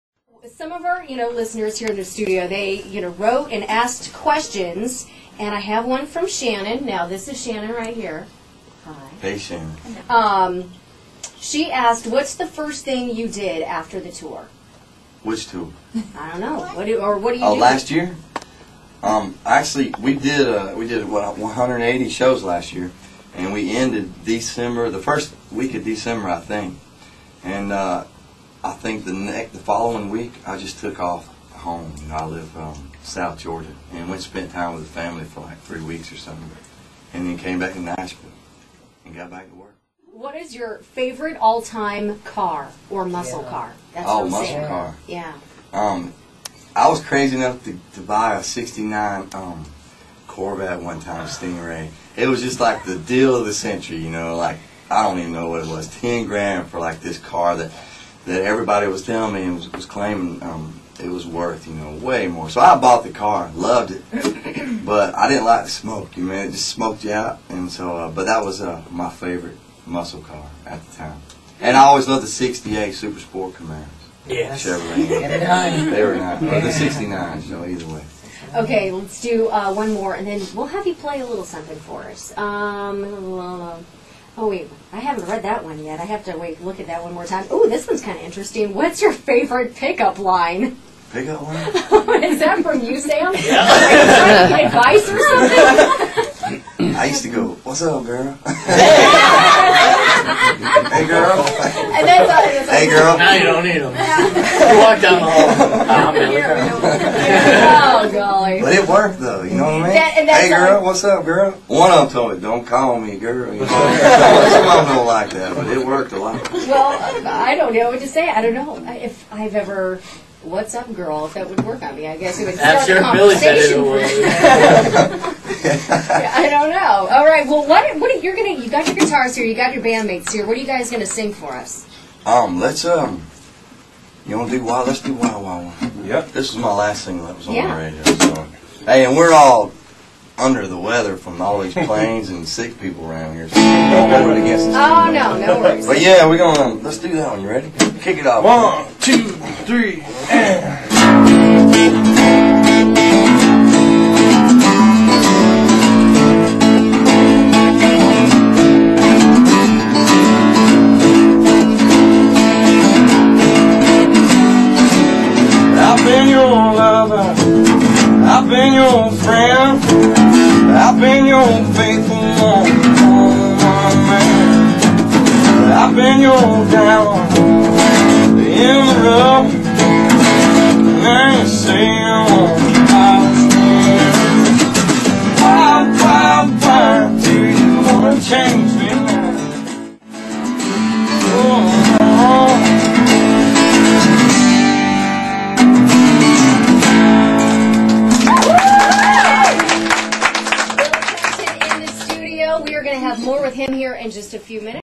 Erin Austin interviews Billy Currington part 2
Erin Austin sits down with the hotty Billy Currington. Listeners get a chance to be up close and talk with Billy. They talk about dating, jobs prior to his big break, his dog, and more! He even sings!